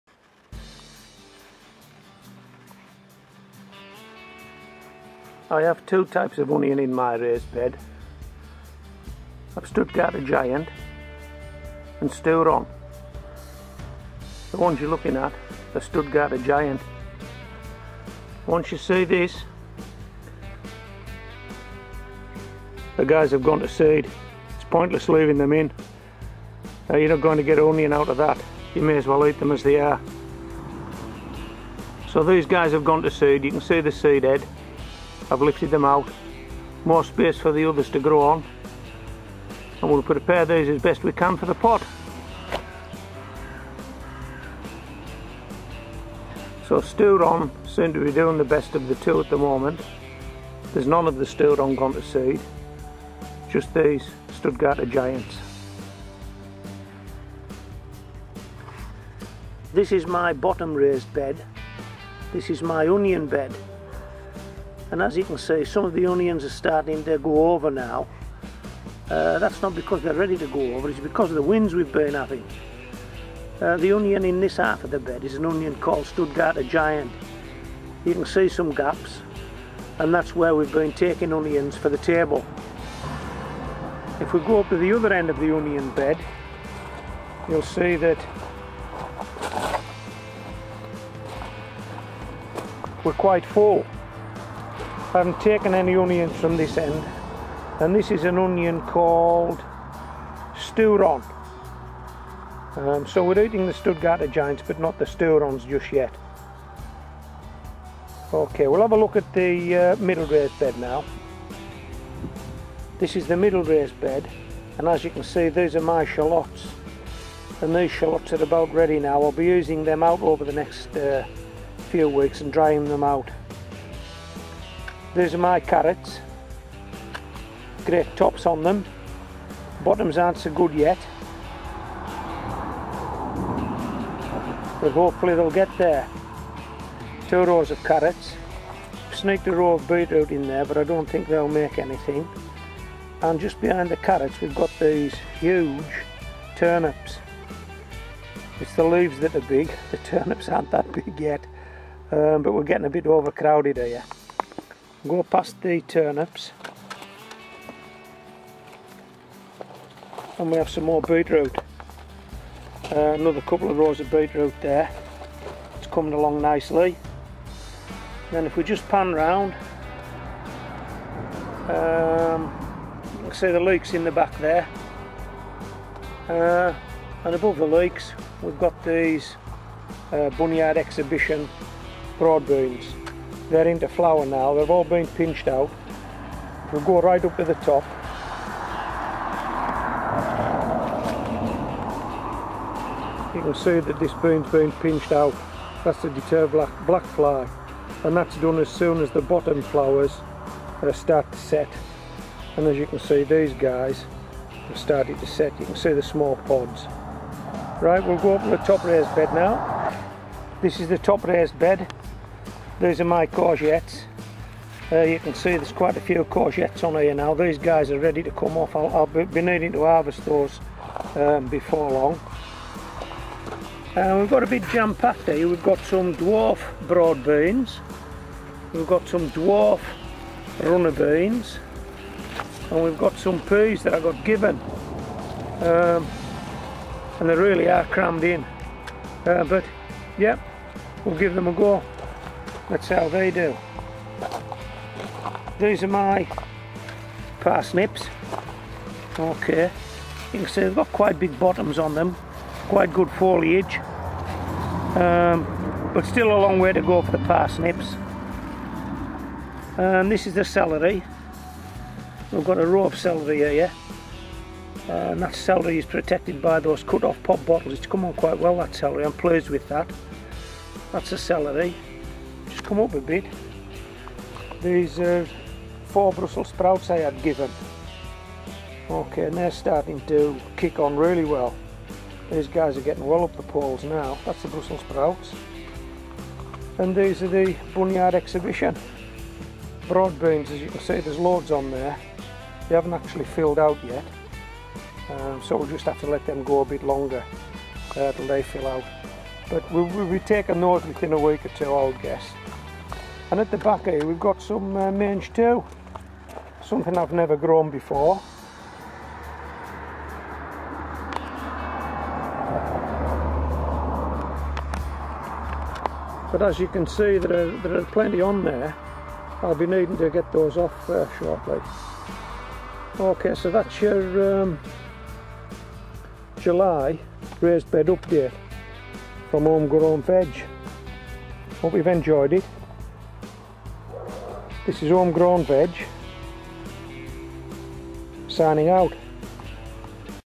July 2015 Raised Bed Update, Home Grown Veg.
All the vegetables in the raised bed have had a bit of a growth spurt this month. Now they are getting bigger and some ready to harvest I can see just how overcrowded my raised beds are. 'We are where we are', so, may the best vegetables win. The cooking pot's waiting.                             This is a FREE subscription channel.